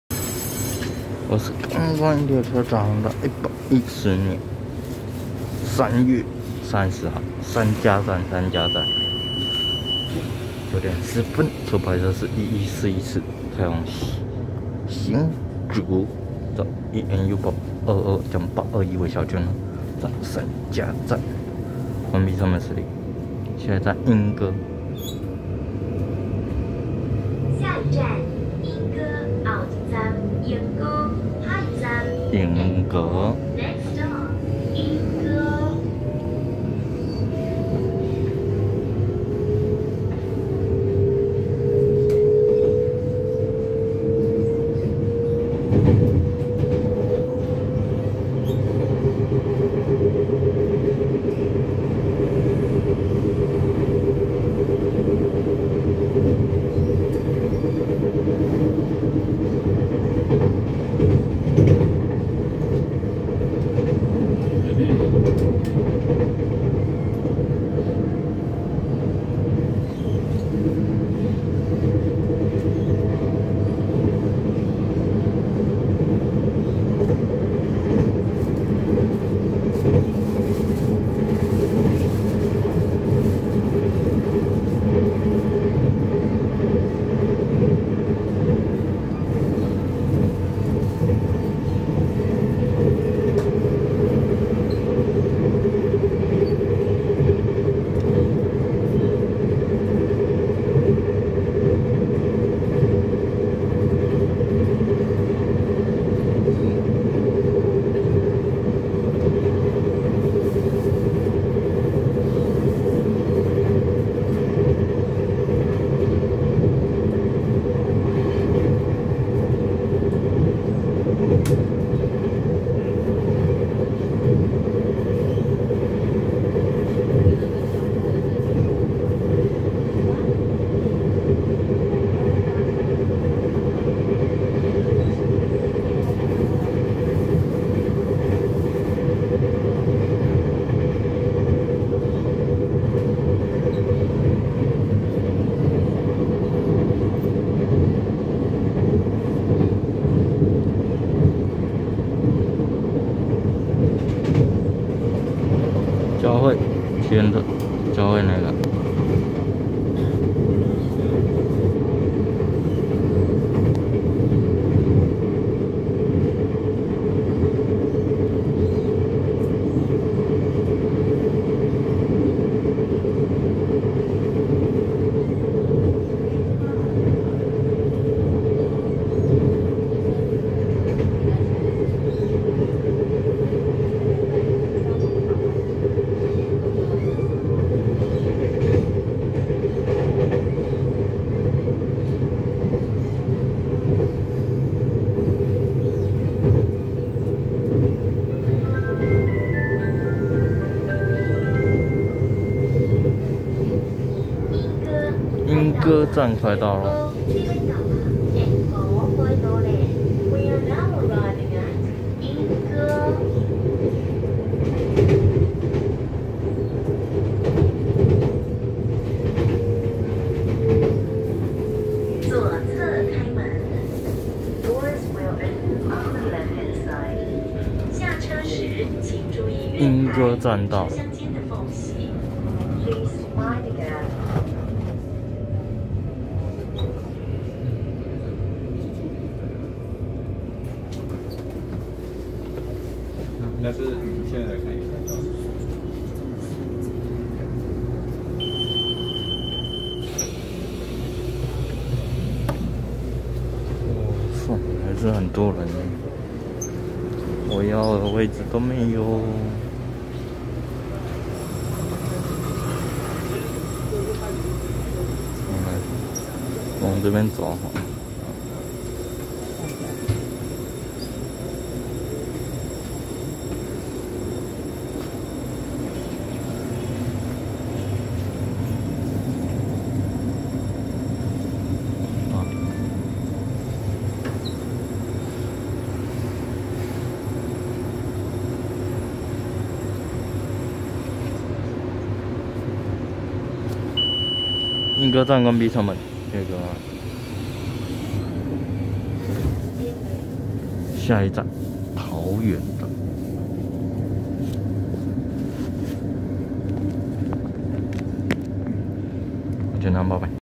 1141次開往新竹的EMU822+EMU821微笑號區間車，(山佳站→鶯歌站路程景)
110/3/30山佳站9:10分拍攝1141次開往新竹的EMU822+EMU821微笑號區間車，(山佳站→鶯歌站路程景)